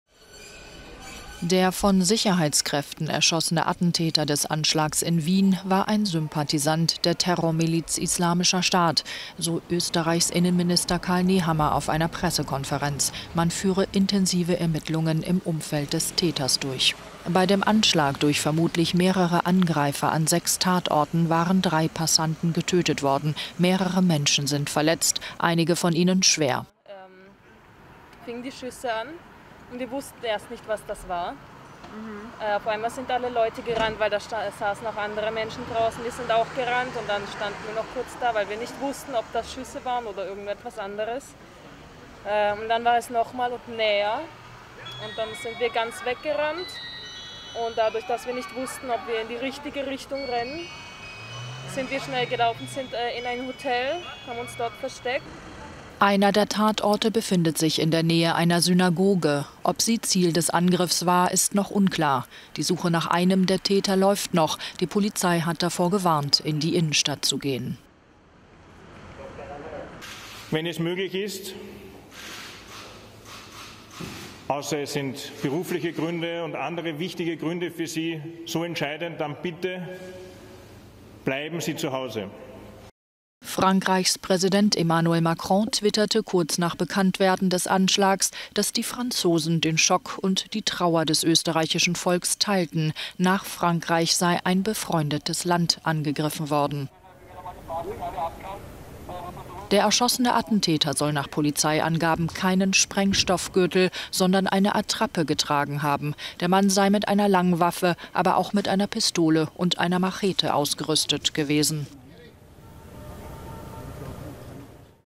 Terroranschlag von Wien "islamistisch" - Täter polizeibekannt
Der von Sicherheitskräften erschossene Attentäter des Anschlags in Wien war ein Sympathisant der Terrormilitz Islamischer Staat